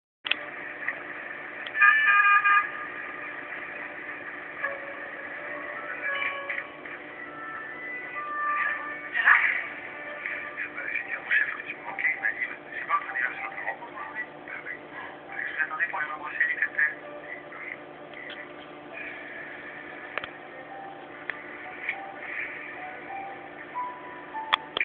10,4 inches LCD display
Old video player + LCD, both connected through S/video.

Framerate is very good, and movie is Xvid.